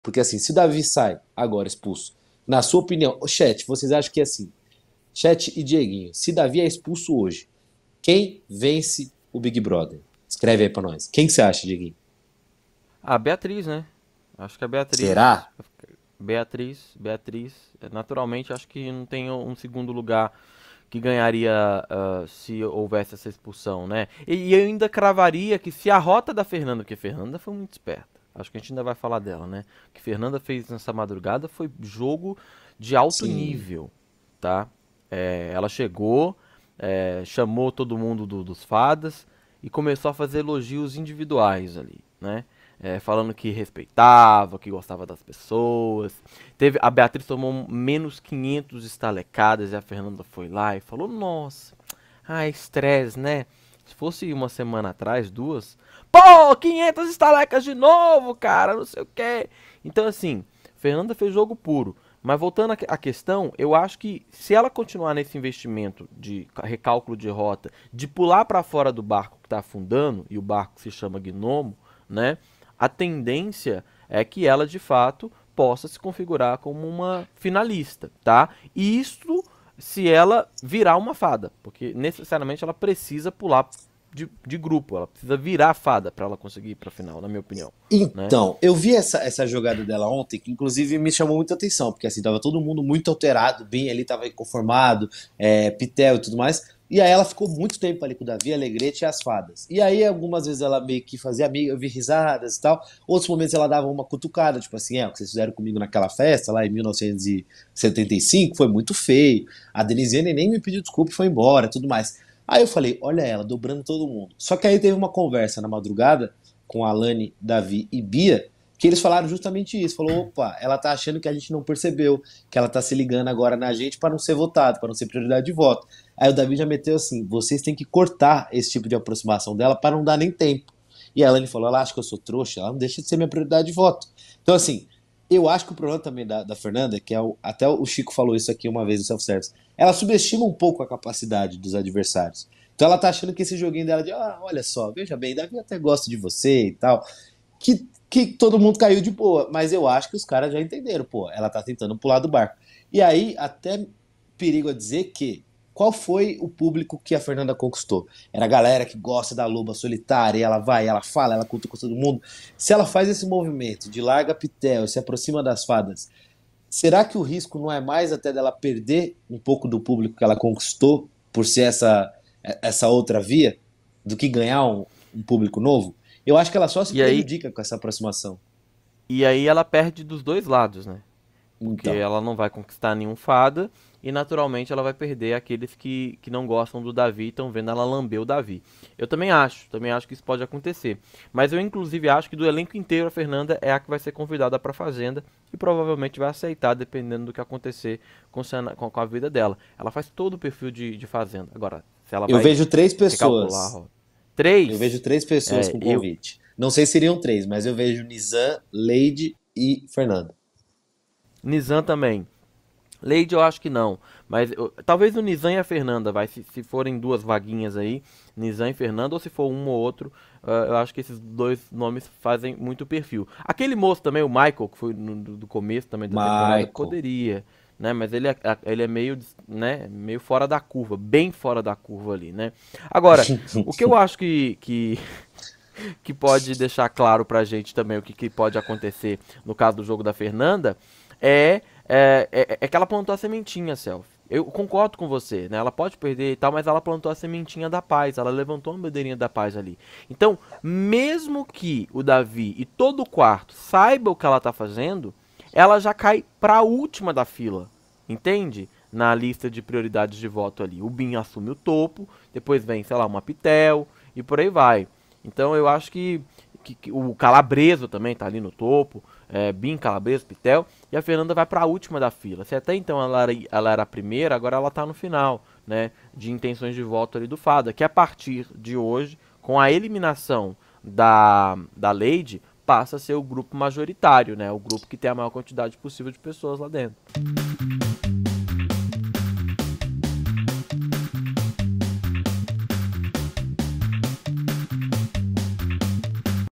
BBB 24: FERNANDA VAI ABANDONAR OS GNOMOS E SE ALIAR ÀS FADAS?
Vista como líder do quarto Gnomos, que uniu os membros do Puxadinho, Fernanda tem conversado cada vez mais com Bia Brasil, Alane, Matteus e até Davi.

Gabi Augusto - @gabiaugusto
Dezza Tornelli - @dezzatornelli
Henrique Mathias - @henriquemathias
Caio Barbosa - @caioobar